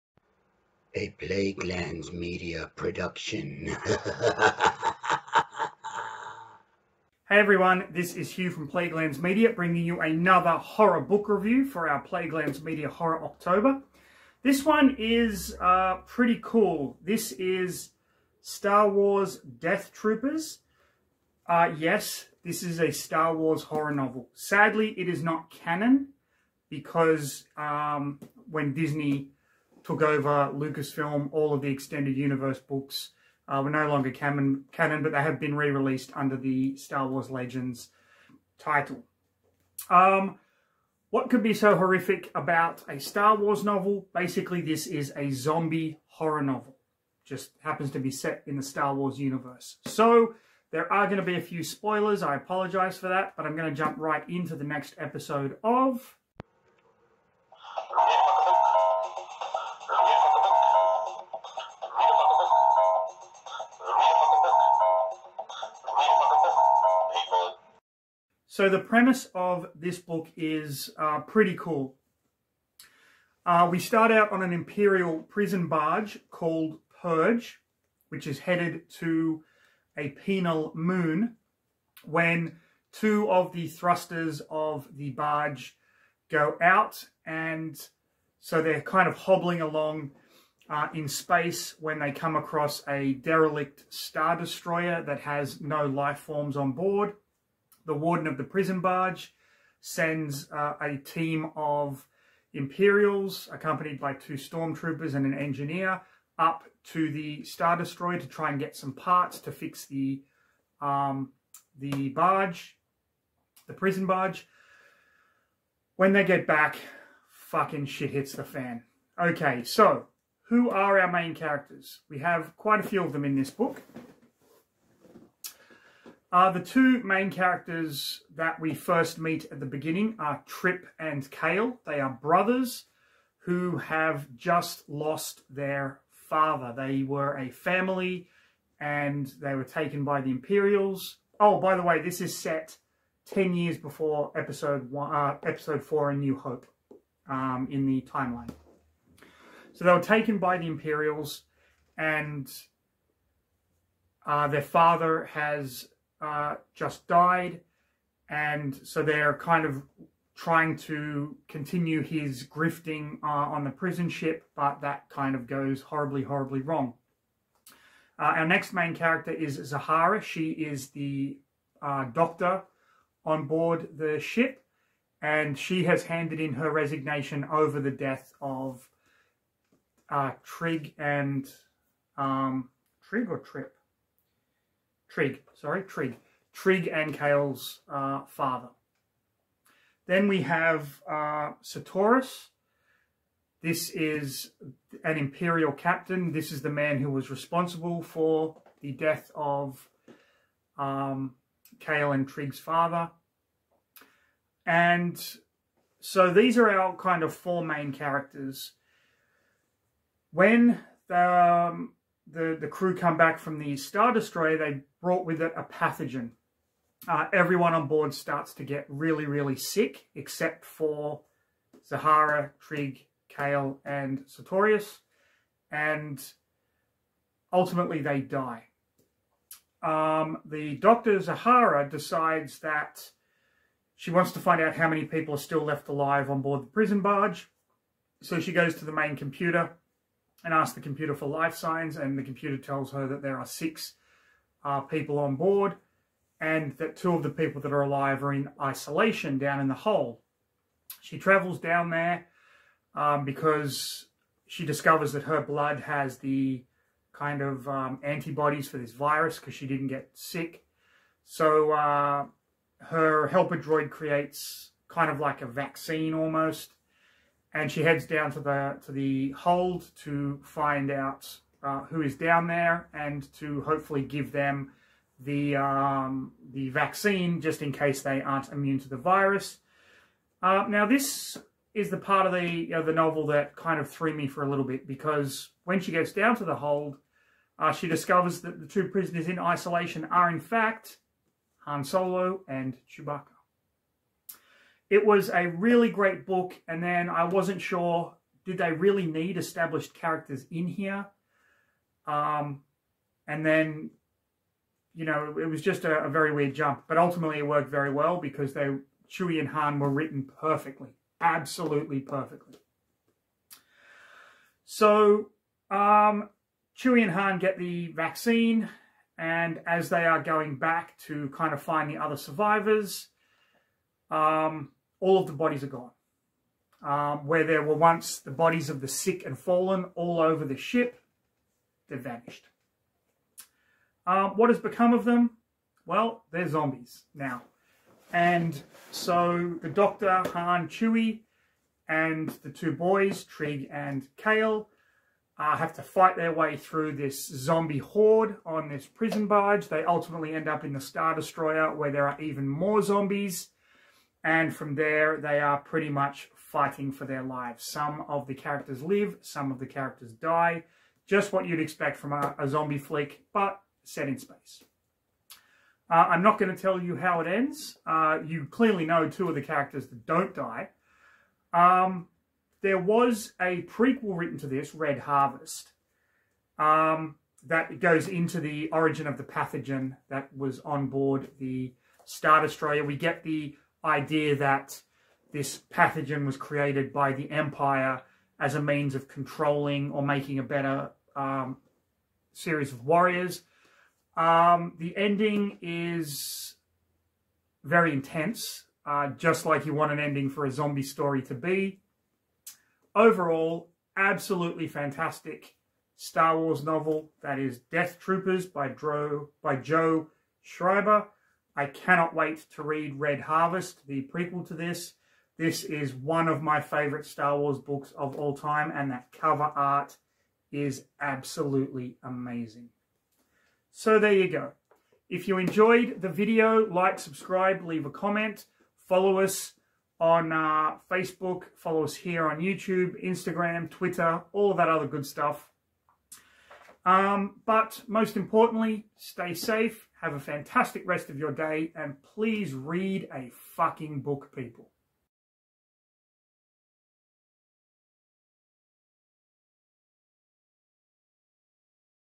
A Book Look - Star Wars Death Troopers by Joe Schreiber
A review of a fantastic Star Wars horror novel.